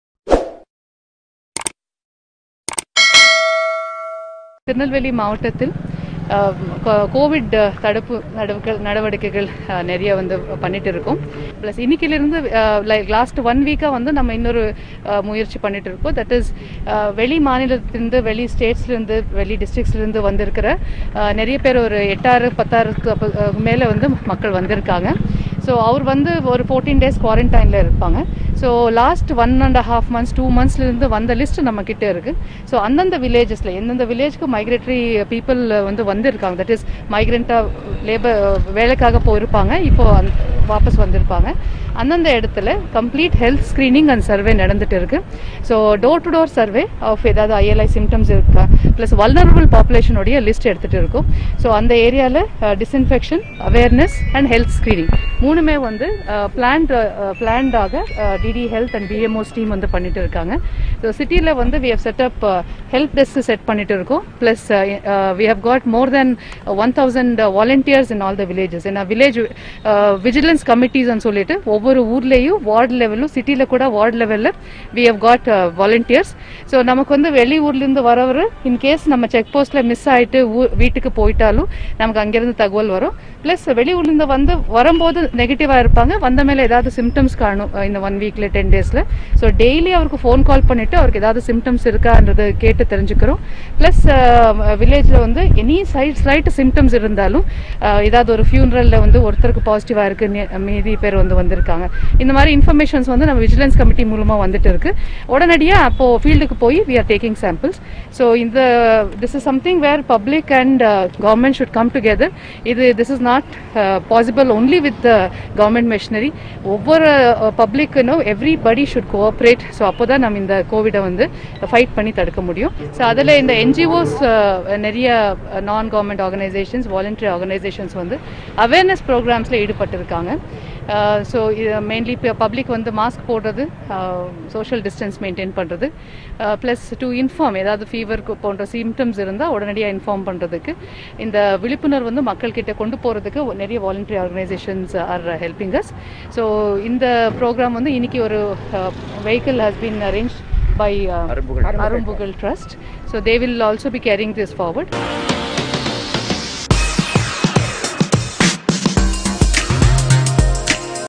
நெல்லையில் குரானா நிலை என்ன? ஆட்சியர் ஷில்பா அதிரடி பேட்டி
நெல்லையில் குரானா நிலை என்ன? ஆட்சியர் ஷில்பா பிரபாகர் சதிஷின் அதிரடி பேட்டி